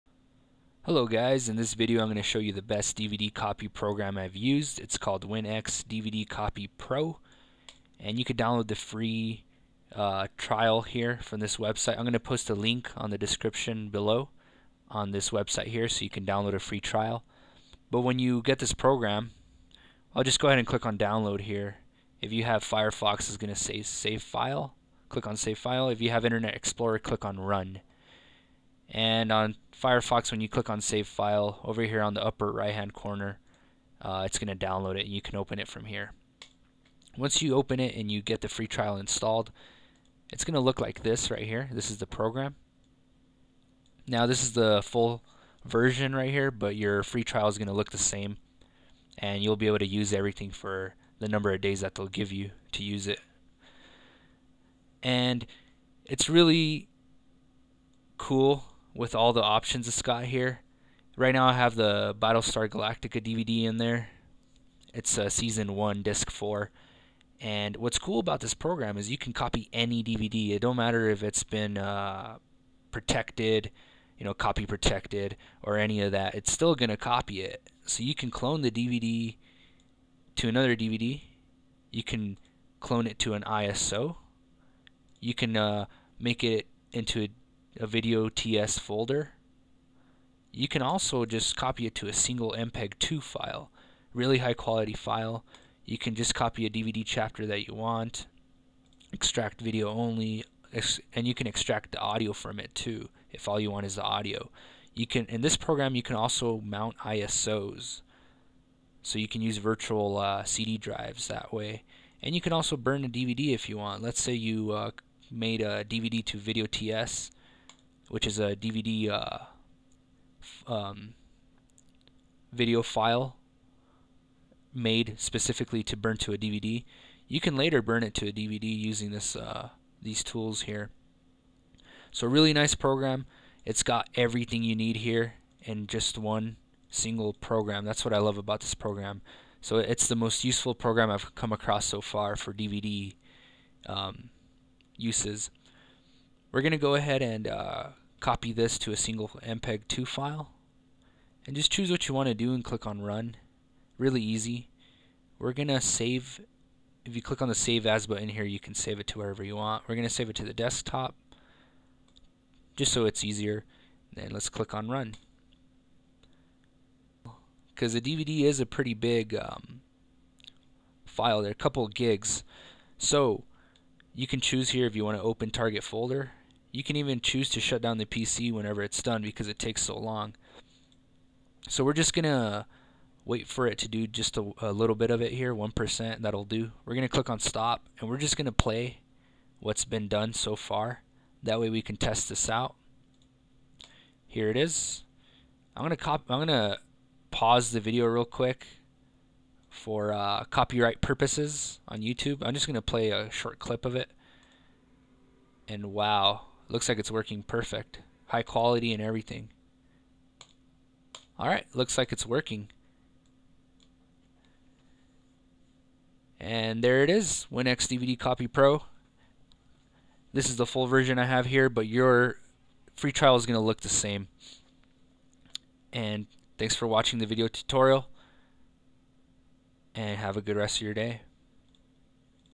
Win X DVD Copy Pro Review
In this video review we will be reviewing the best DVD copy software I have used so far.  its called Win X DVD Copy Pro.  With this program you can clone any DVD, even copy-ptotected DVDs.  You can also clone to ISO, Copy to Mpeg2 file, VOB file, and it also has the option to mount ISOs and burn DVDs.

tune-up.